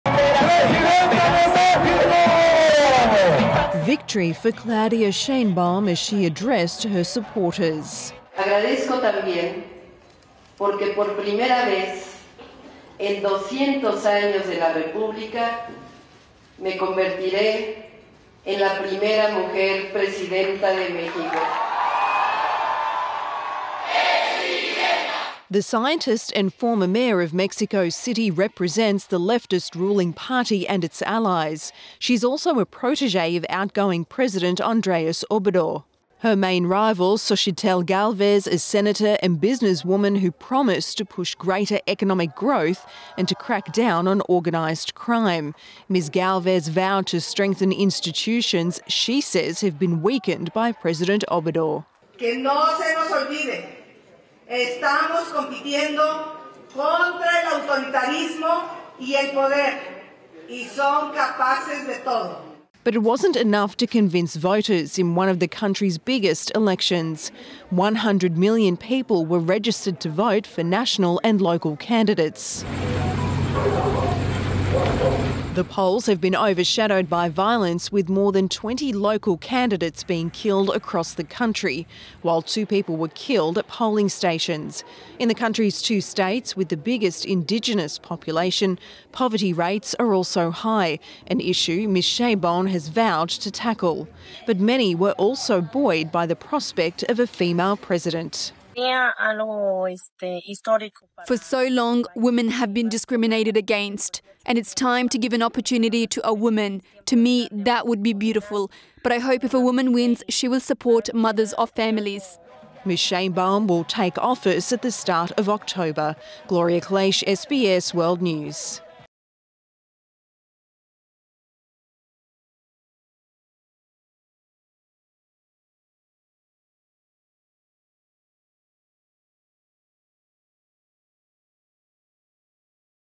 Mexico has elected its first female president Claudia Sheinbaum
Ruling party candidate Claudia Sheinbaum won Mexico's presidential election, according to the INE electoral institute's rapid sample count released Sunday night.